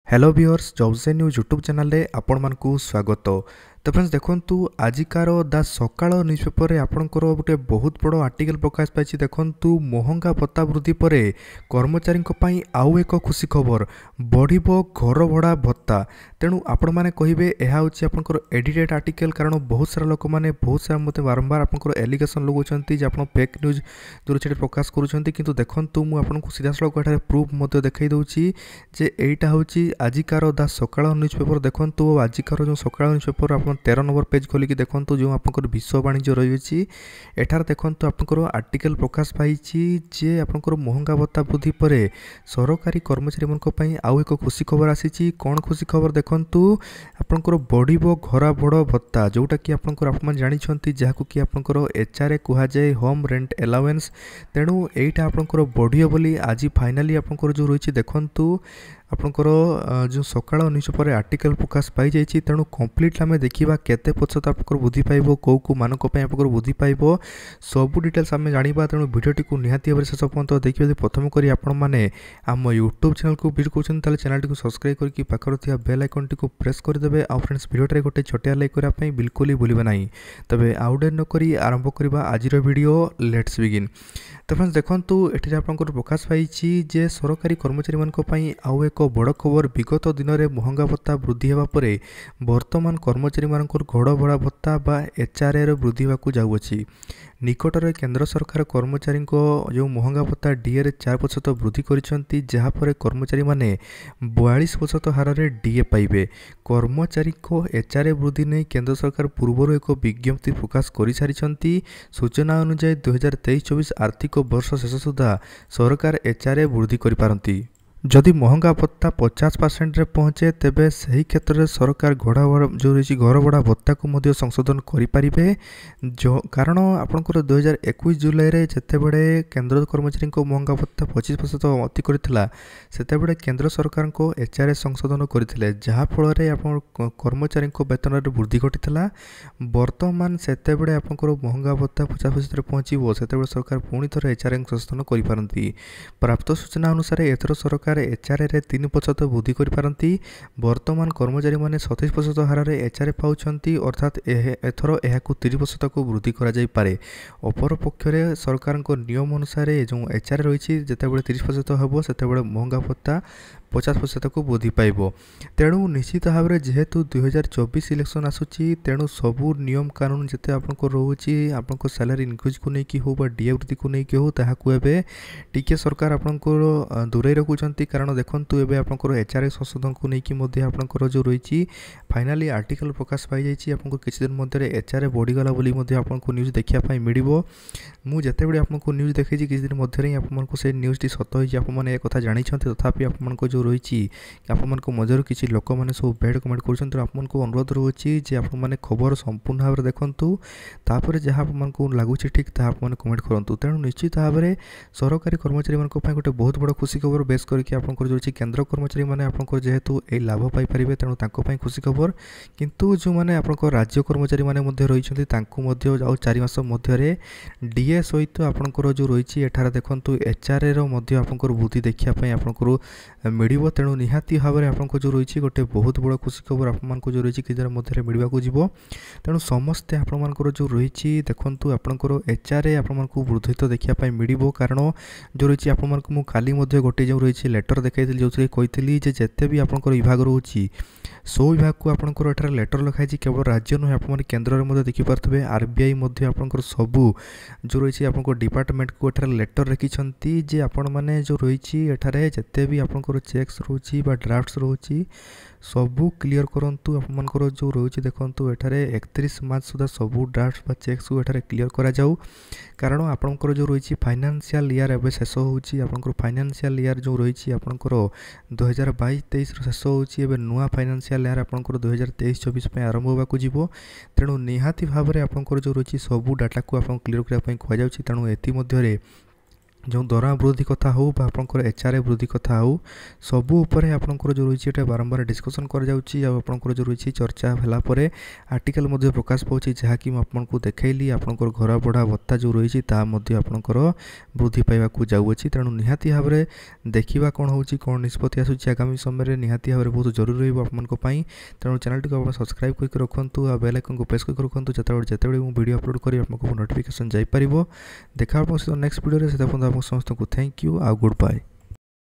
🌟DA ବୃଦ୍ଧି ପରେ ଏବେ 30% HRA ବୃଦ୍ଧି, 2024 ପାଇଁ ସରକାରଙ୍କ ତରଫରୁ ସବୁଠୁ ବଡ ଭେଟି // SALARY INCREASE ODISHA
Jobs & News And RSP Web Solution